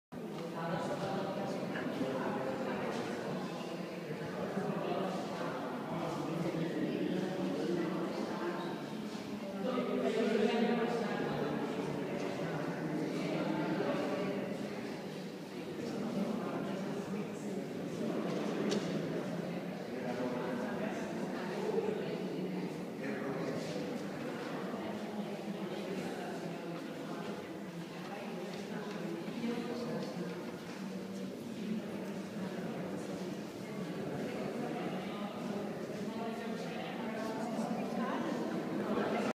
Tàpies + Augmented Reality 2
Augmented Reality included in the more complete exhibition in the world of the artist Antoni Tàpies. The museums Fundació Miró and MNAC in Barcelone launched the exhibition together last 21 of june, in memory of the first aniversay of the died of the artist.
You can use the application for mobile devices MusGUIDE and LAYAR to see the paintings with Augmented Reality in the exhibition.